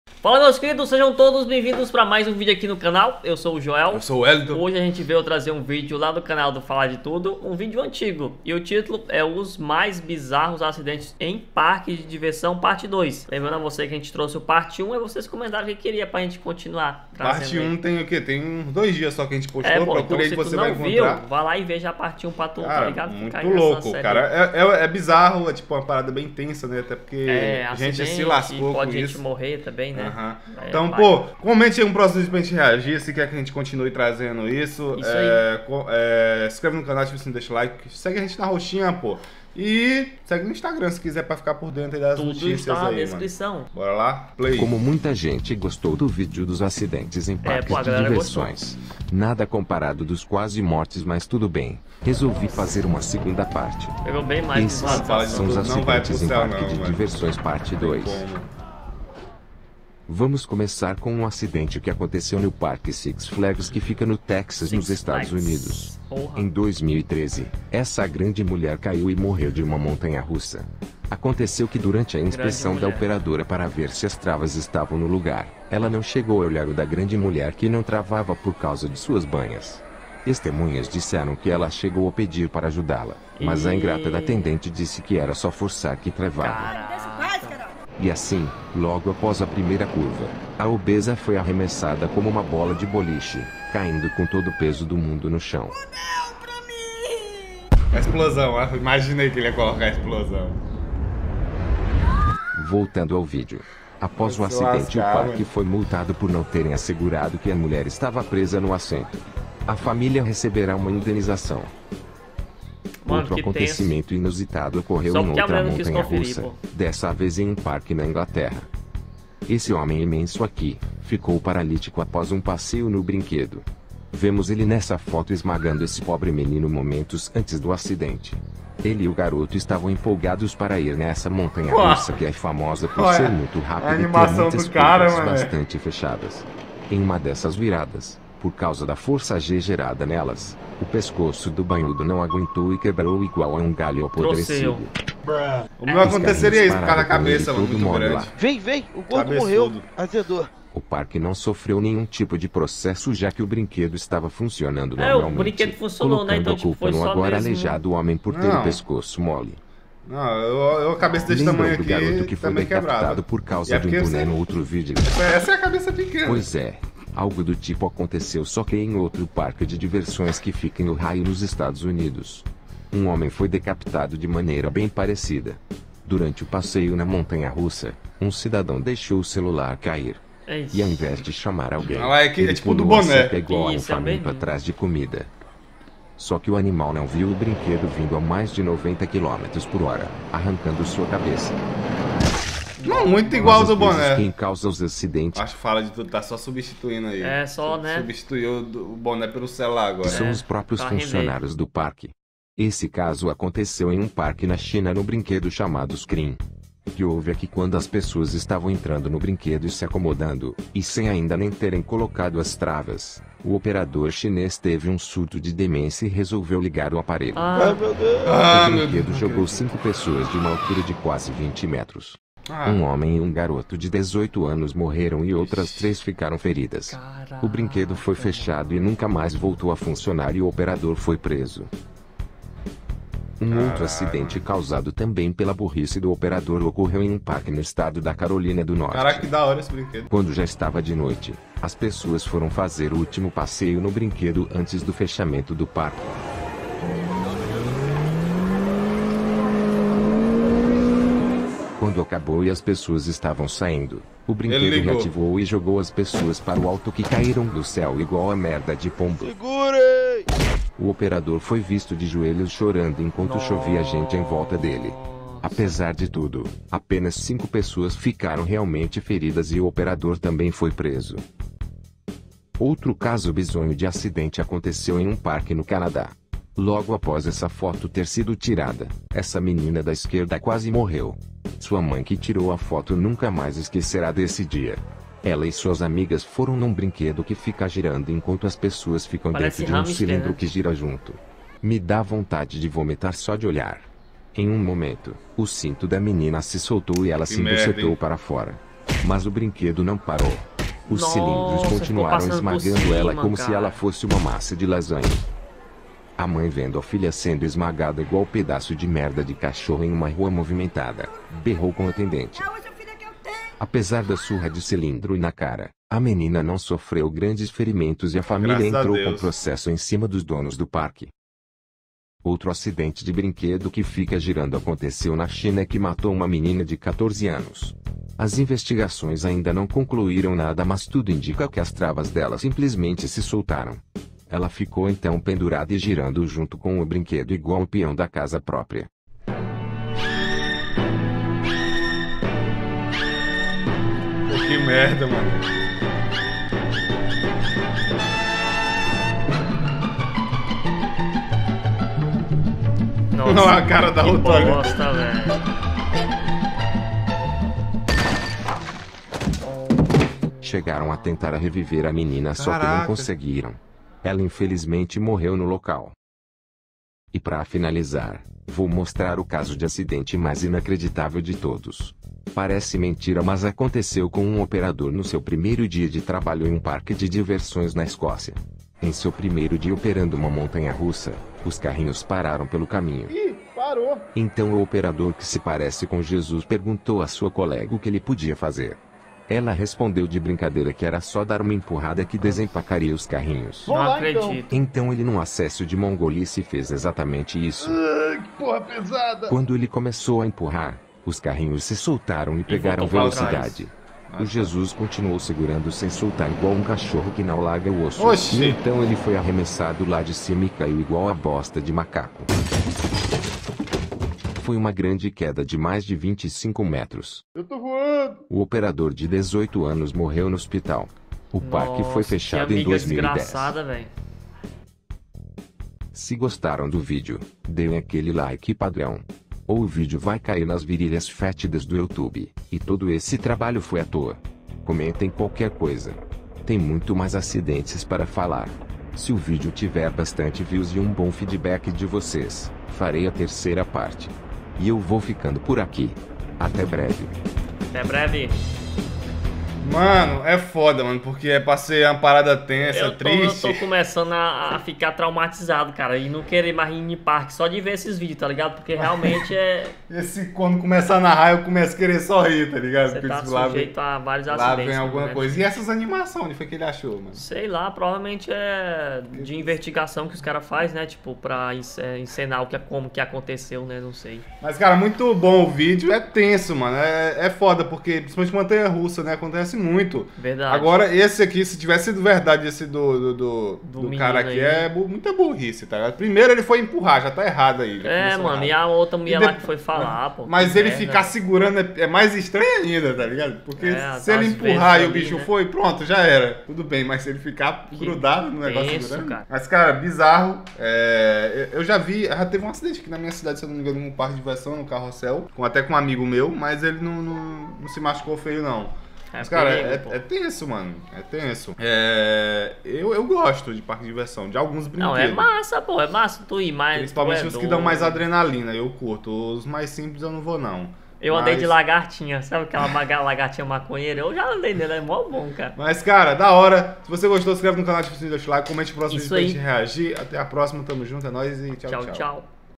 REACT 🔴 Os mais BIZARROS acidentes em Parques de Diversões! PARTE 2 (FALA DE TUDO)
Fala meus queridos, como é que vocês estão ?
Hoje reagimos a um vídeo lá do canal - Fala de Tudo

Canal de lives na Roxinha: periodico_lives

Você nos motiva a continuar :)